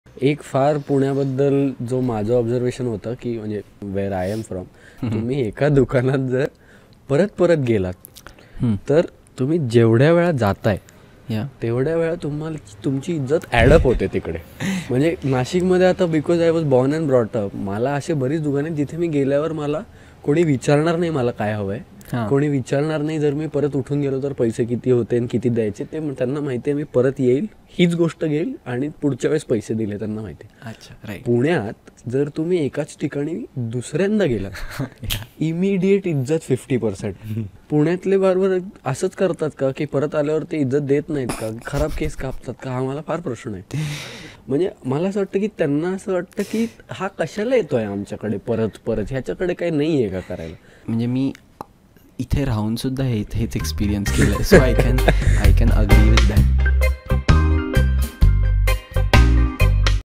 Nashik Boy vs Pune Barbers!
In this fourth episode of 'Koffee with Kia', Sohira & Krushna our boys from Nashik & Jalna have a heart-to-heart with Samar about e-Commerce, UPSC, 'Moving out', B. Liberal Arts and everything in between. Even Pune Barbers!